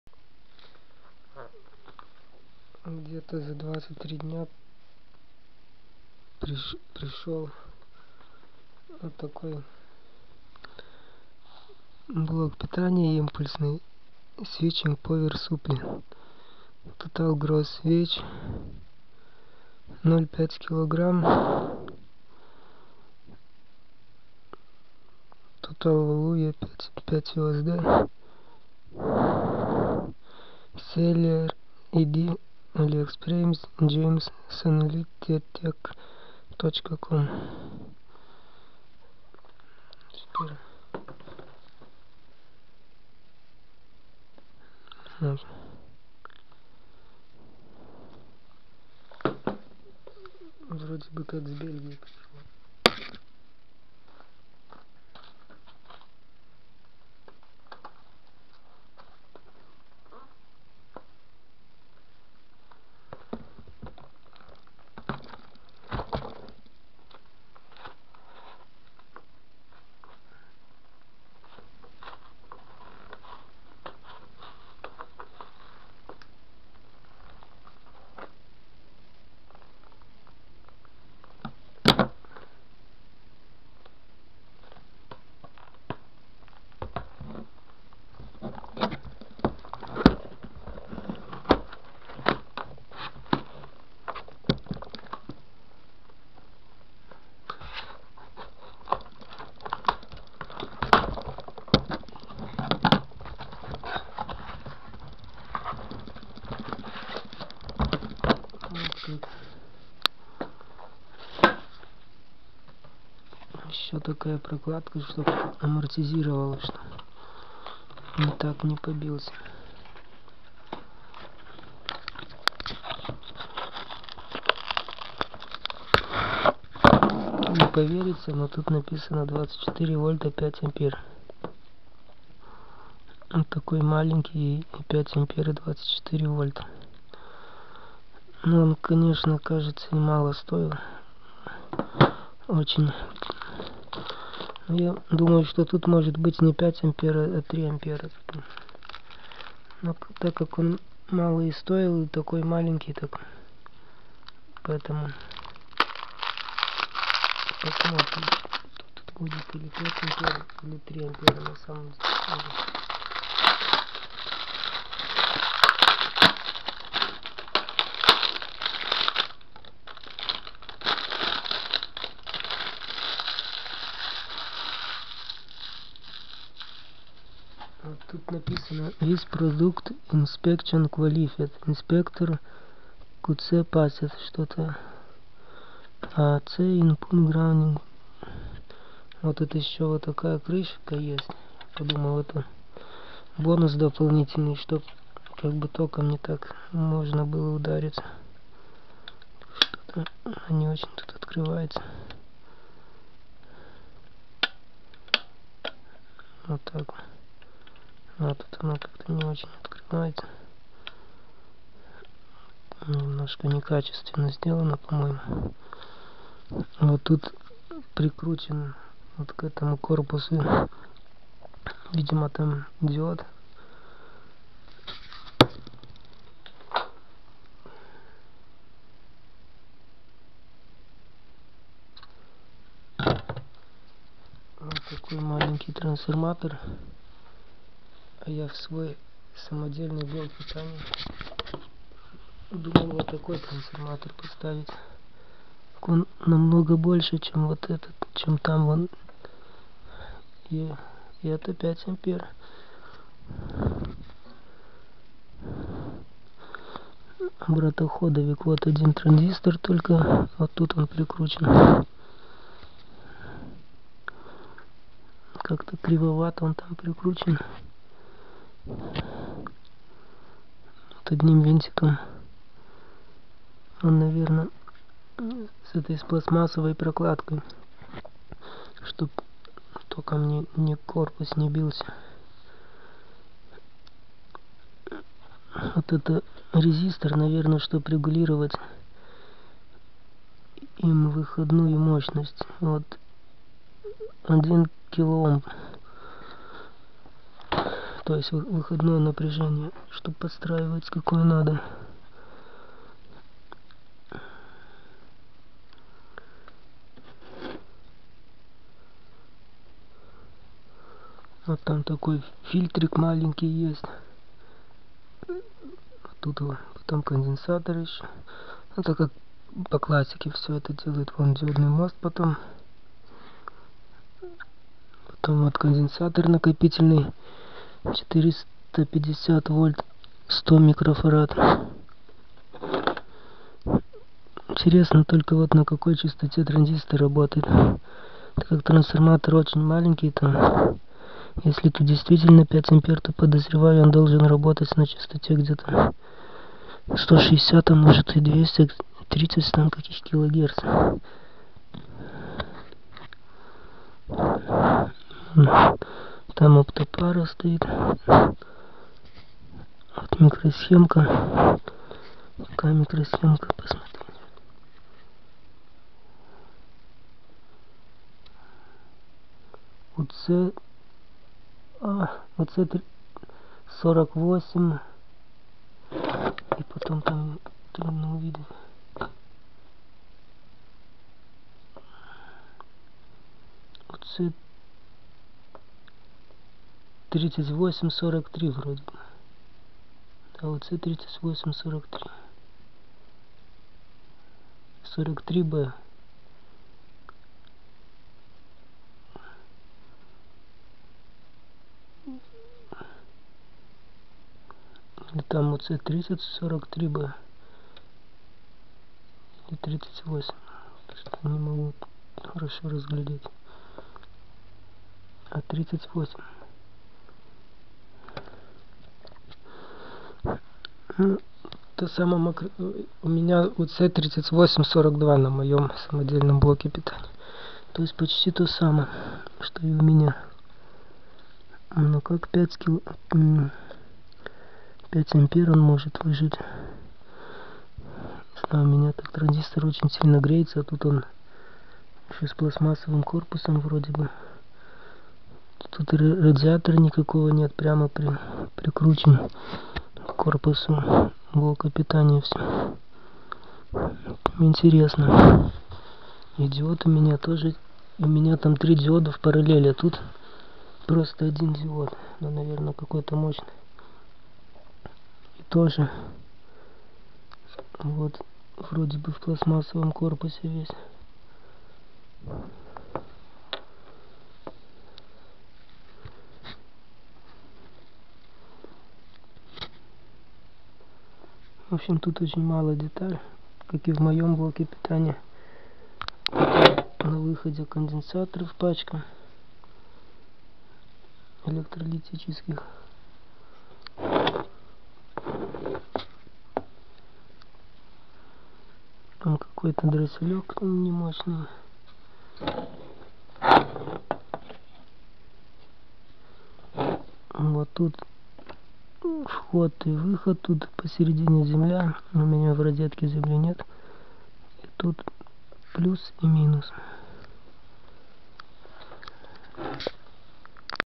Посылка блок питания 24 В 5 А за 12,4 долара.
Сылка на его

Когда работает, какой-то шум есть у него небольшой при нагрузке. Не очень мне этот шум нравиться похоже на какие-то проблеммы. Или может это после качера так стало...

Не очень стабильное напряжение. Без нагрузки 24,2 вольта. С нагрузкой 5 ампер 23,8-23,9 вольт.